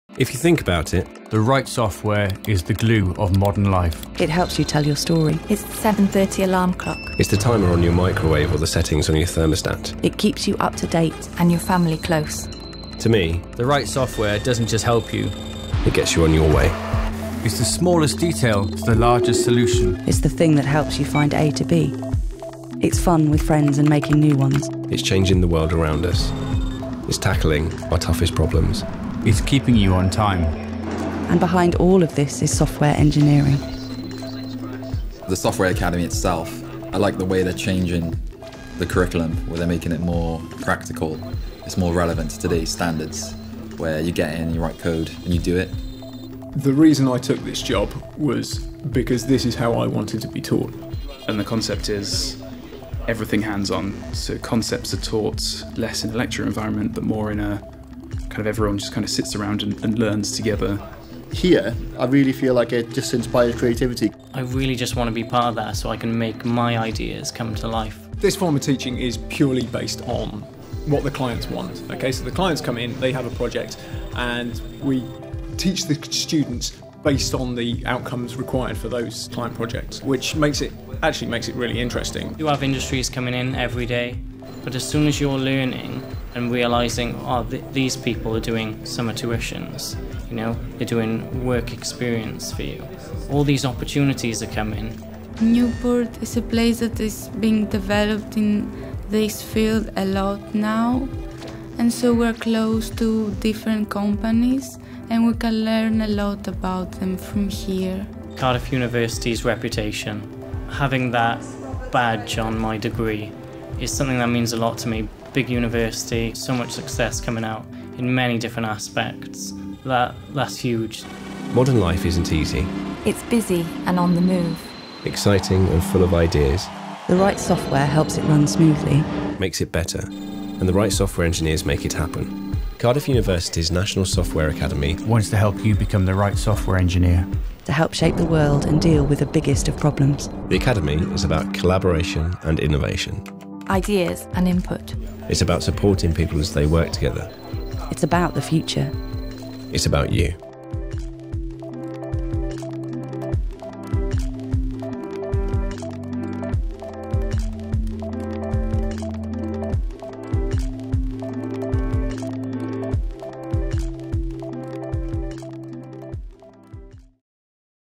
National Software Academy in Newport Wales
The National Software Academy is an exciting new initiative by the University, working in partnership with Welsh Government and industry leaders, which aims to address the national shortage of skilled programming and software engineering graduates.

Based at The Platform, a new technology hub in Newport, the National Software Academy will deliver an innovative degree with a focus on the skills, knowledge and hands-on experience required to be immediately effective as a commercial software engineer.